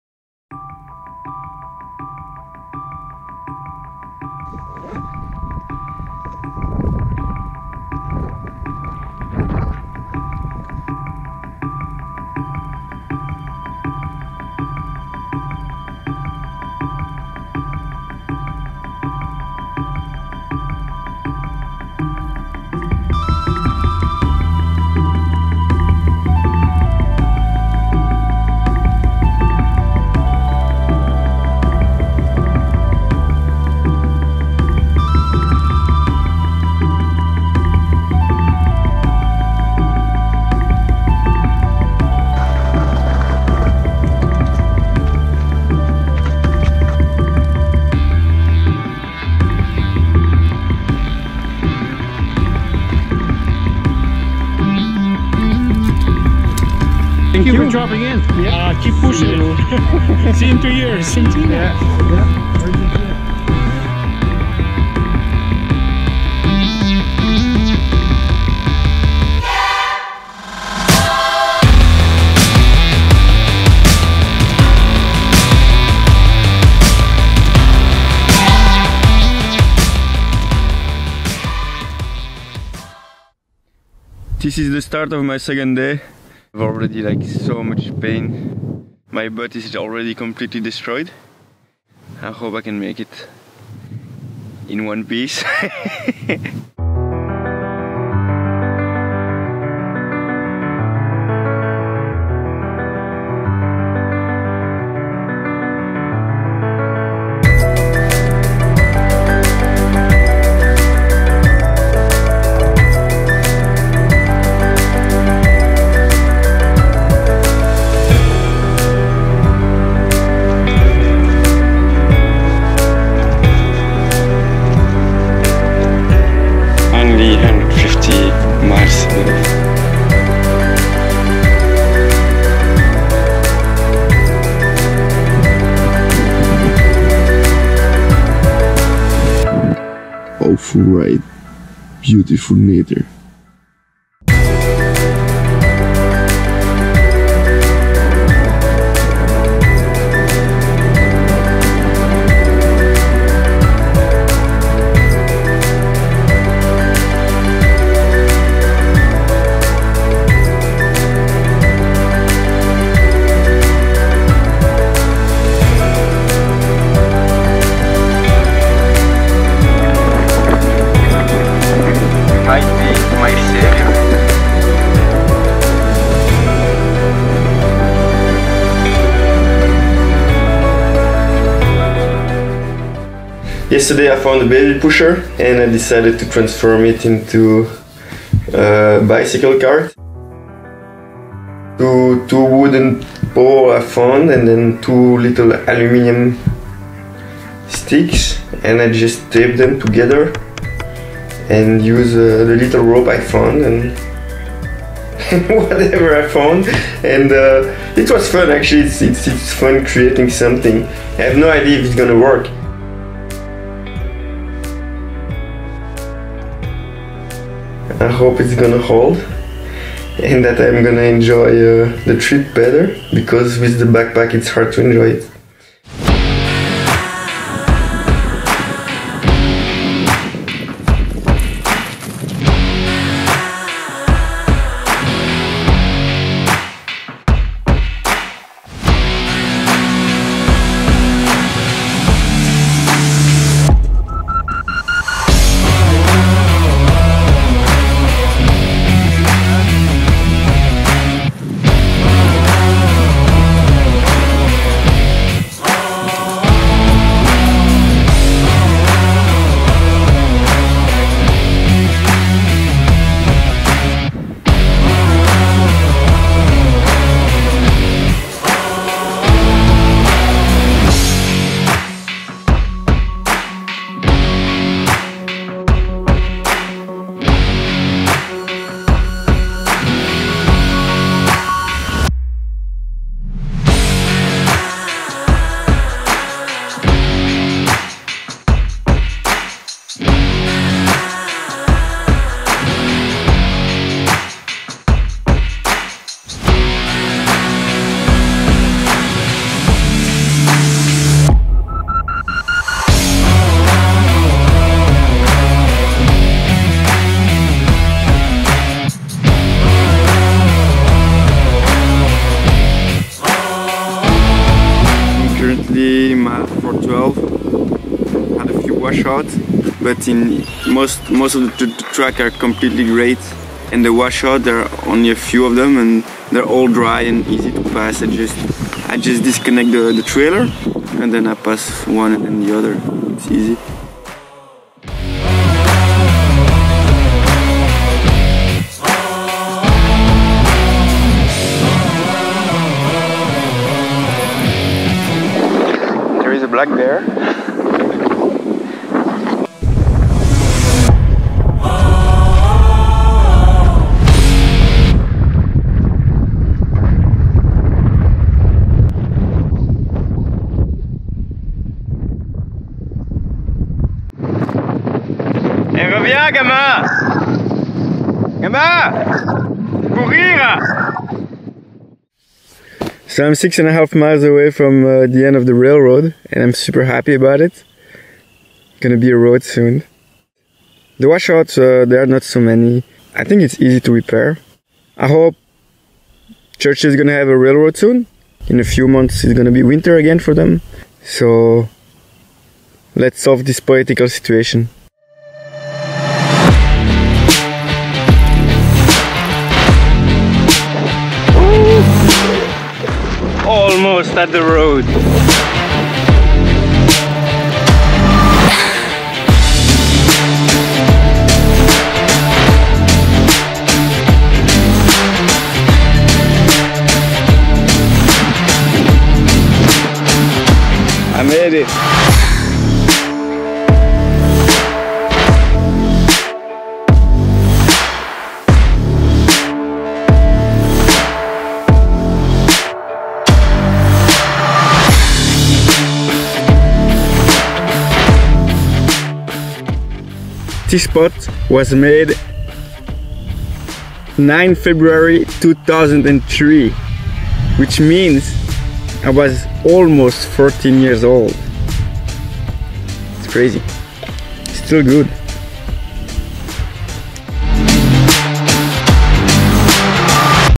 The Railroad
Follow the adventure :
Livexplorer.com/themannekentrip

After skiing the Arctic for 100 days, I arrived in Churchill, Canada, only to discover the city had no more functioning railroad. The rails were flooded on 23 may 2017 after a huge winter storm hit the region earlier this winter. The damaged rails suffered some washouts, which cut the city only ground supply and communication mean.

Private owner Omnitrax, whom is legally bind to maintain the tracks,  refused to repair the line, pretexting exaggerated costs and financial failure. The government refused to funnel money to the company, resulting in a political drama and no repairs. Churchill's citizen are stuck with high prices, jobs cuts and a bitter feeling of being abandoned. The situation also impacted the whole Kivalliq region, Nunavut, which relied on Churchill rail supply line.

One year later, nothing had changed...

Since the rails were part of my itinerary and "The Manneken Trip" expedition, I decided to shoot this video while cycling the rails down towards Gillam and later Winnipeg. The idea was to generate some awareness and report on the state of the rails.

As expected, the damages aren't that bad, and could easily be repaired. It was a horrible ride with its lot of nice surprises! Nature was super beautiful however : the taiga, the boreal forest and lots of birds. Three days after finishing the trip, 41 communities joined together with private company Fairfax and AGT in order to buy the Hudson bay railroad and port. It's an historic move from these community which retransfer ownership into local hands!

However, no date has been set for the repairs yet... Due to intensive and long winters, repairs can only take place during the few summer months. If repairs don't start soon, Churchill might have to face another winter without train.

Arnaud Maldague.

More stories on the facebook page, instagram, youtube channel and blog: themannekentrip.be
You can also follow the expedition live on: Livexplorer.com/themannekentrip